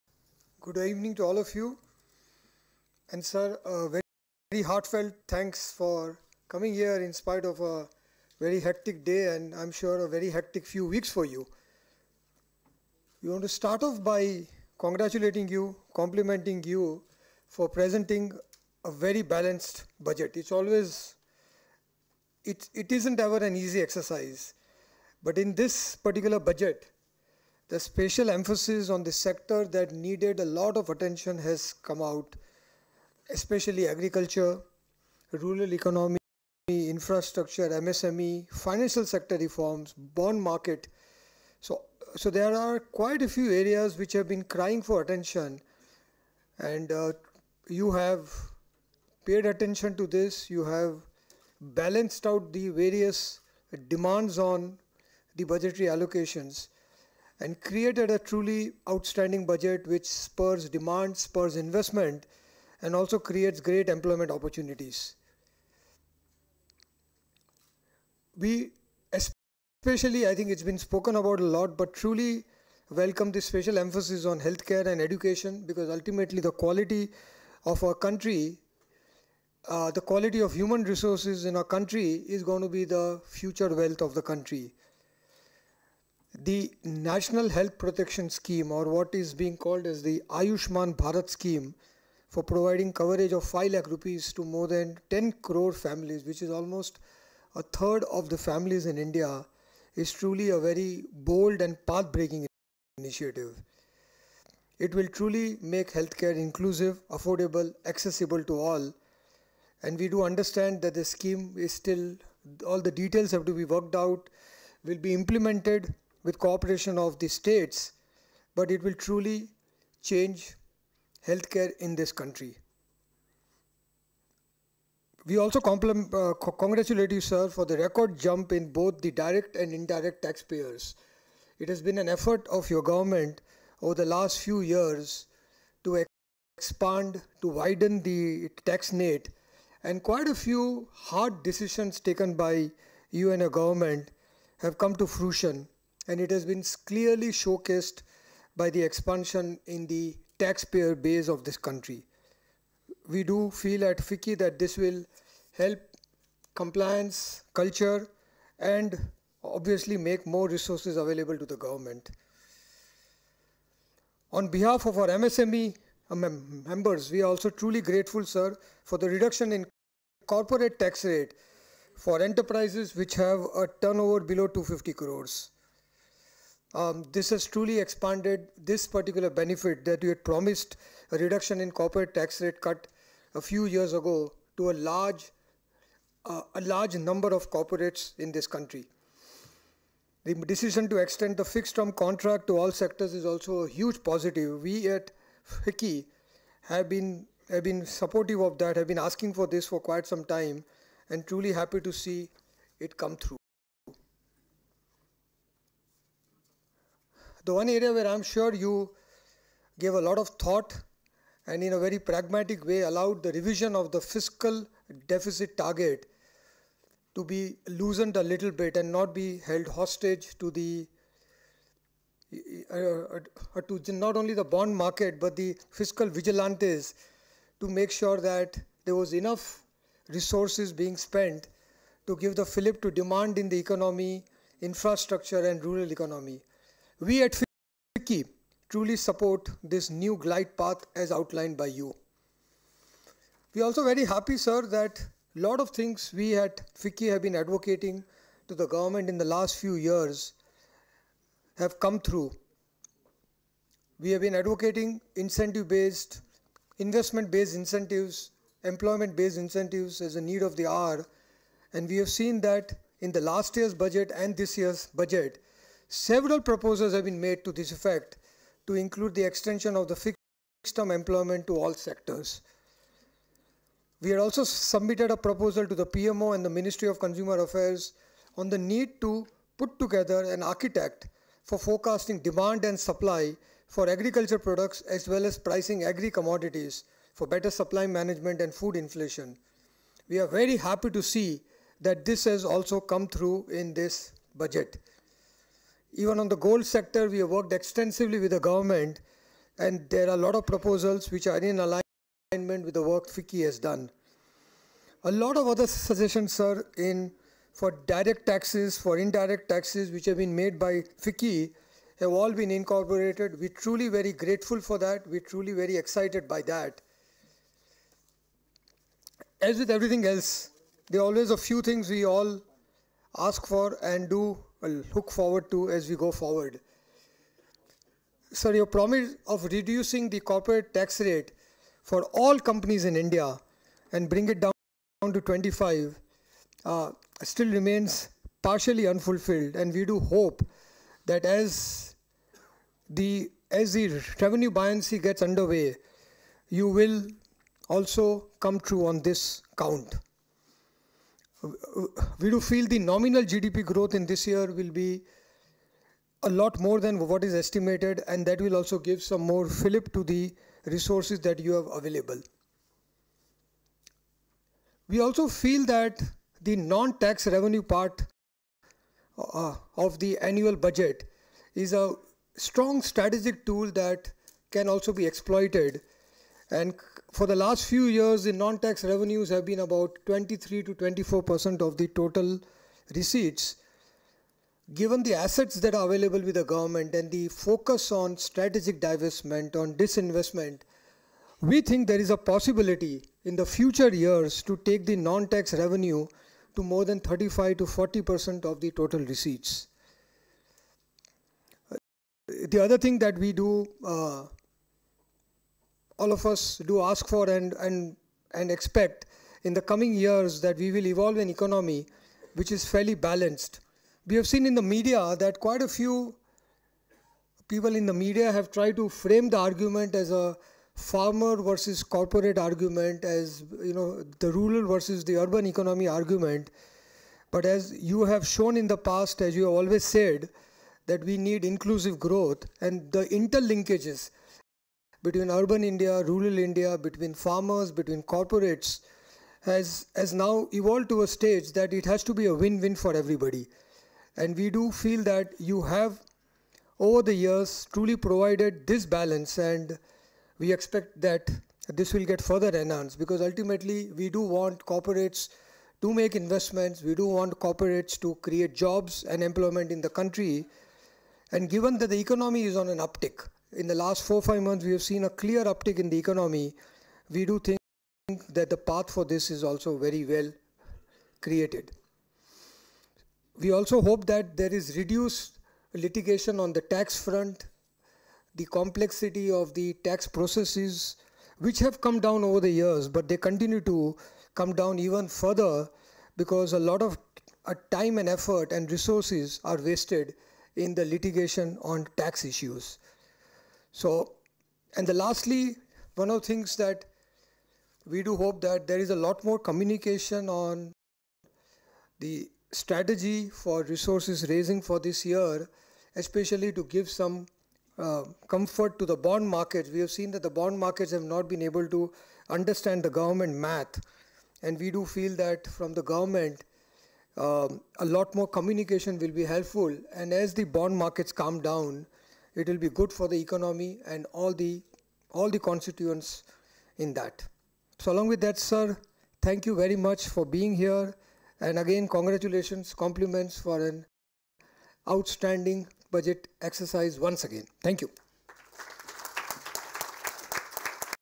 Rashesh Shah, President FICCI at NECM
Rashesh Shah, President FICCI, giving his welcome address at our National Executive Committee Meeting